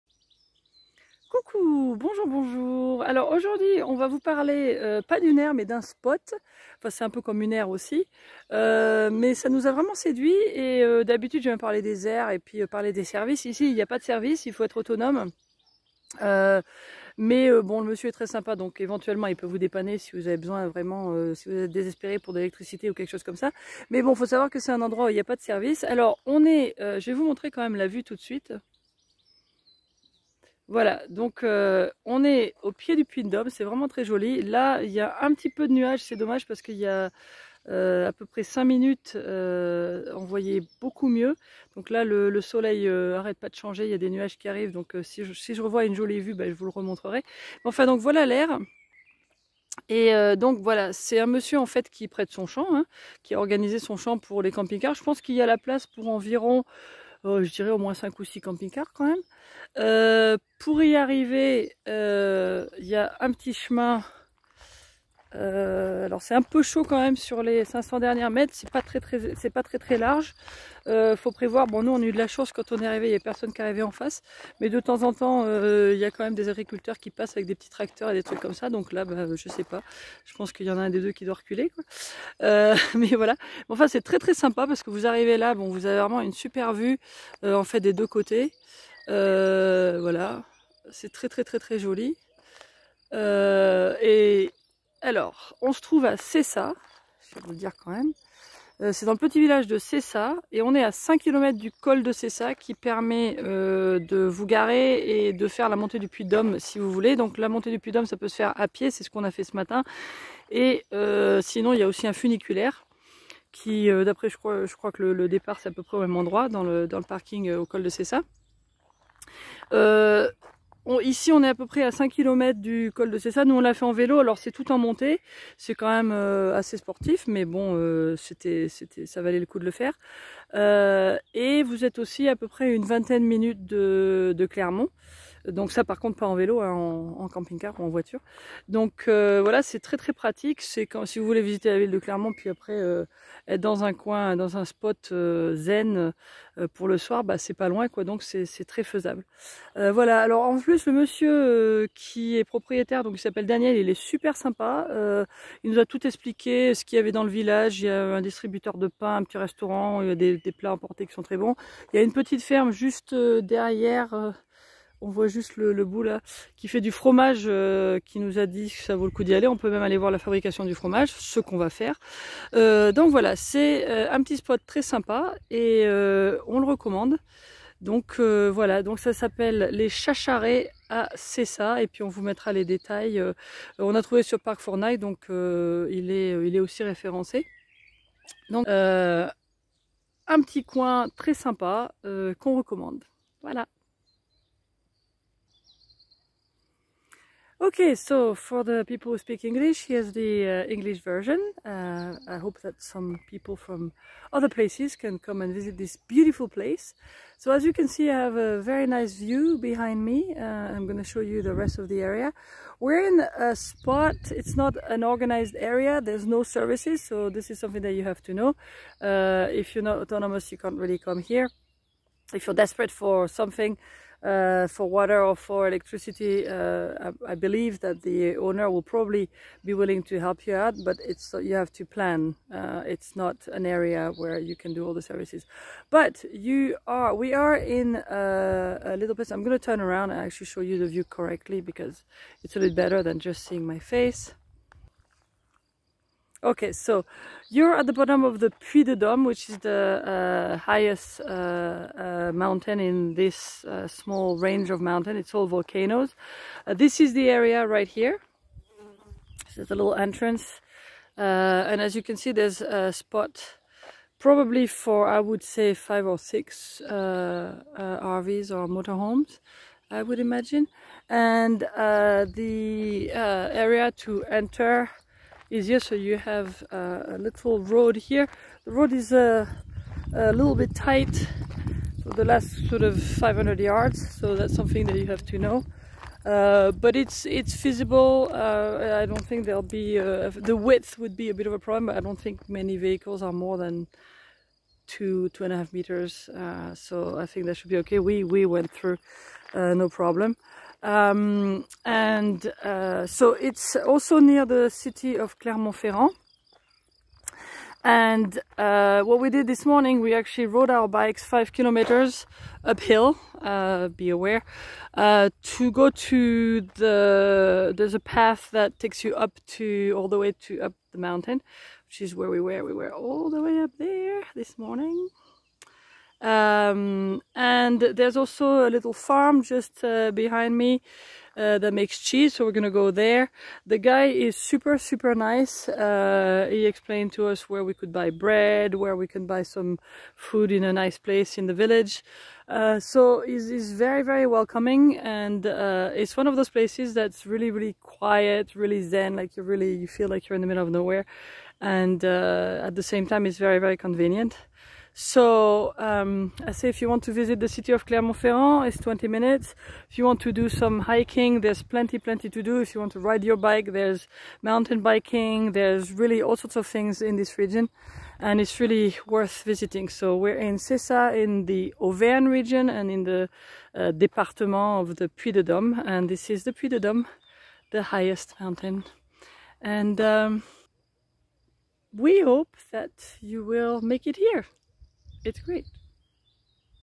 Aire de Ceyssat Les Chacharets
Attention, il est possible que cet endroit ne soit ouvert qu'une partie de l'année, ou bien qu'il n'existe plus maintenant (Mars 2023).

English version: 3'17"
Nous voilà dans le Puy-de-Dôme en Auvergne, dans le petit village de Ceyssat. Une aire d'accueil gratuite pour campingcaristes, sans services de vidange ou remplissage, au pied du Puy de Dôme. Daniel, l'adorable propriétaire du lieu viendra vous souhaiter la bienvenue et vous indiquera tout ce qu'il y a à voir, faire, manger, découvrir dans les alentours. Notez que les poubelles sont à quelques minutes à pied dans le village.
À 5km du col de Ceyssat où vous pouvez monter au Puy de Dôme à pied ou en funiculaire. Clermont-Ferrand n'est qu'à 20 minutes en voiture/c.c. Et une super vue sur le Puy de Dôme. Super endroit.
Adresse ci-dessous. L'aire est aussi sur Park4night et Caramaps. Comme il n'y a pas beaucoup de places, il vaut mieux appeler avant. Détails dans le lien Park4Night ci-dessous.

Here we are in Puy-de-Dôme in Auvergne, in the small village of Ceyssat. A free area for motorhomes / RVs without services, at the foot of the Puy de Dôme. Daniel, the very friendly owner of the place, will come to welcome you and tell you all there is to see, do, eat and discover in the surroundings. Note that rubbish and recycling are a couple of minutes walk down in the village. You'll be 5km from Col de Ceyssat where you can climb to  the top of Puy de Dôme on foot or by funicular. Clermont-Ferrand is only 20 minutes by car / c.c. And a great view of the Puy de Dôme. Great place.
Link above. The area is also on Park4night and Caramaps. As there are unfortunately not many spots, it is better to call before. Details above in the Park4Night link.